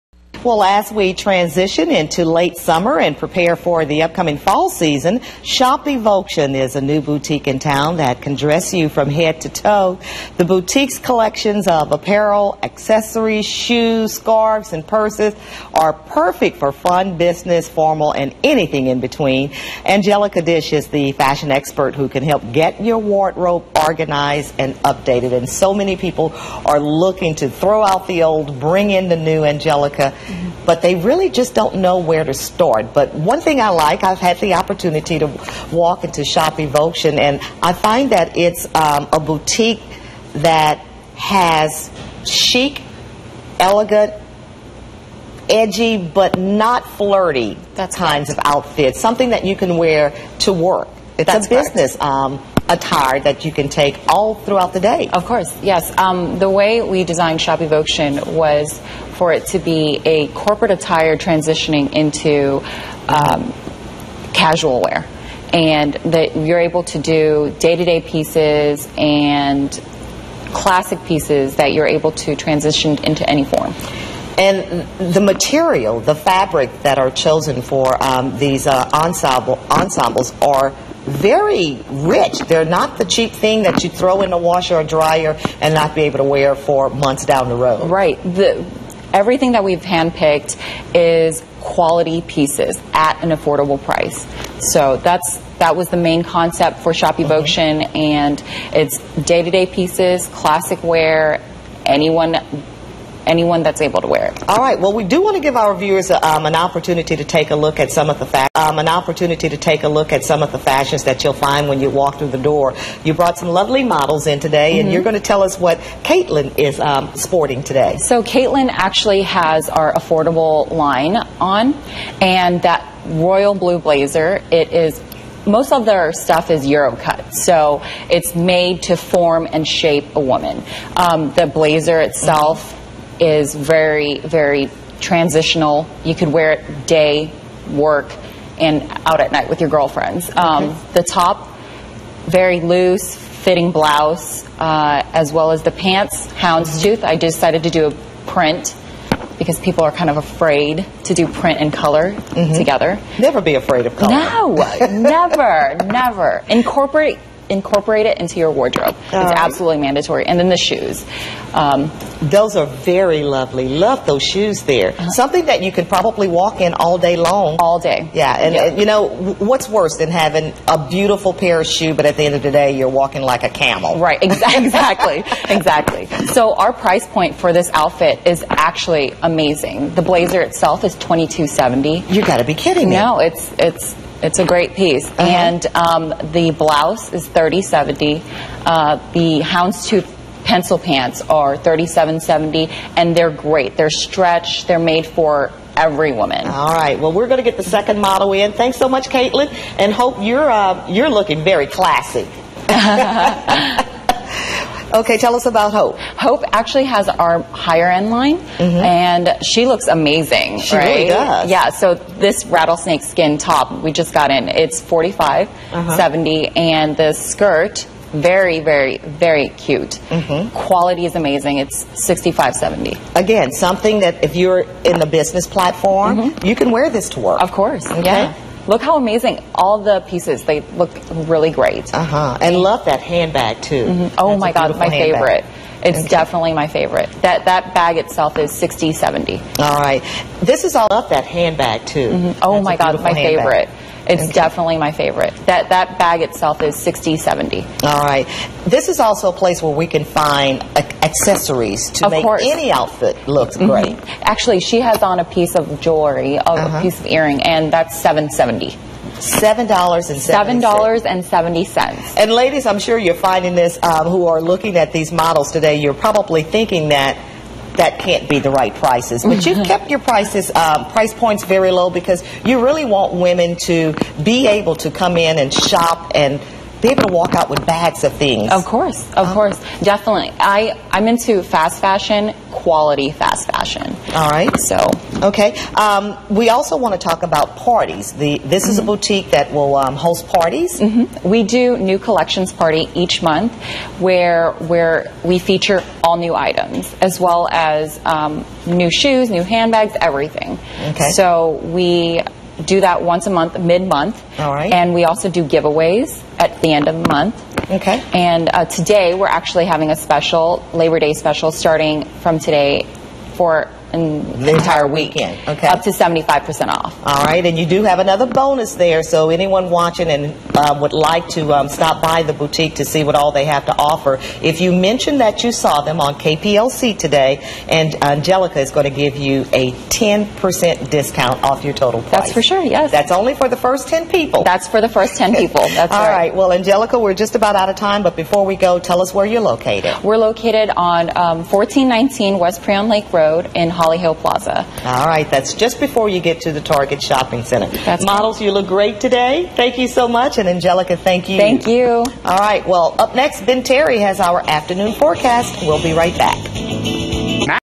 KPLC & SHOPEVOKTION: shop trendy classic pieces at an affordable price!
KPLC features SHOPEVOKTION for a midday snack! Come. shop and have a drink with us at our new boutique in the sweet little town of Lake Charles, LA.

LOCATION:
Holly Hill Plaza
1419 W PRIEN LAKE RD.
LAKE CHARLES LA 70601